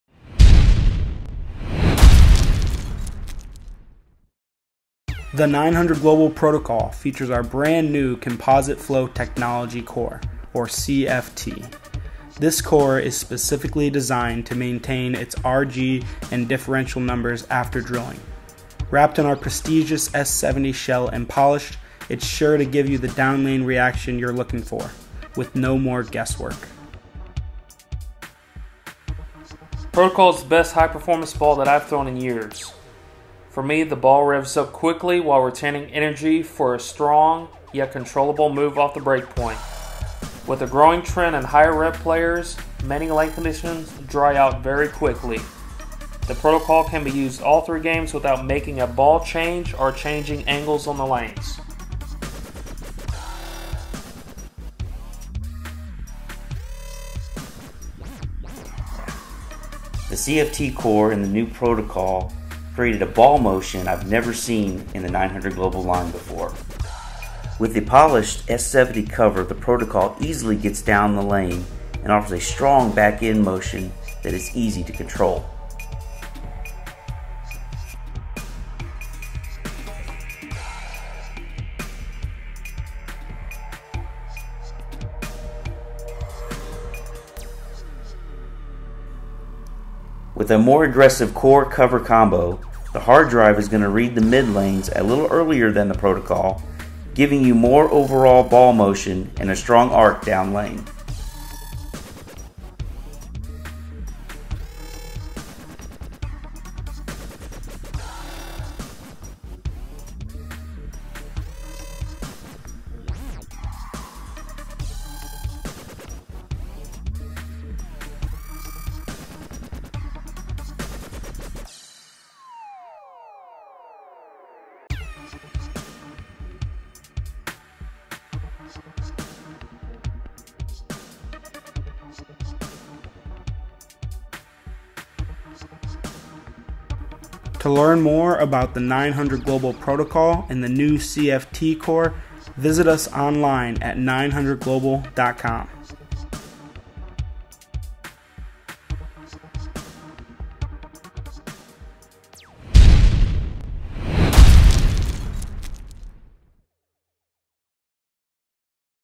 900 Global Protocol bowling ball video
Introducing the new Composite Flow Technology Core (CFT) Featured in the NEW 900 Global Protocol...

Utilizing Fluid-Flow Design Tactics and Composite Material Flow Principle of Chemical Engineering, we've created something truly unique; the first dynamic core to feature Mass Property Stabilization.  But what does this mean for you?  After drilling, the asymmetric CFT Core maintains its designed Rg and Differential values, allowing bowlers to get exactly what they want and expect out of their ball.

When evaluating bowling balls for your bag, you may often look at the Rg (Radius of Gyration) and Differential number associated with a particular core.  Understanding these values helps you relate the expected strength of the core and type of ball motion to your specific style.  Unfortunately, what you don't see is how these numbers change after actually drilling the ball.  Based on the ball layout chosen, certain areas of the inner core are removed during drilling, altering the core dynamics away from their designed values.

The CFT Core was designed specifically to maintain differentials that fluctuate in other designs due to drilling.  With the CFT Core, the Rg and Differential numbers advertised are the core numbers you'll get when you throw the ball.  That mean NO MORE GUESS WORK.

The CFT Core showcased in the new High Performance Protocol features a moderate Rg of 2.53 matched with Differential value of 0.042 and a strong Mid. Differential of 0.023.  These numbers, combined with our prestigious S70 Coverstock and polished will provide a clean skid through the fronts with a very pronounced, aggressive backend flip down-lane on medium-heavy lane conditions.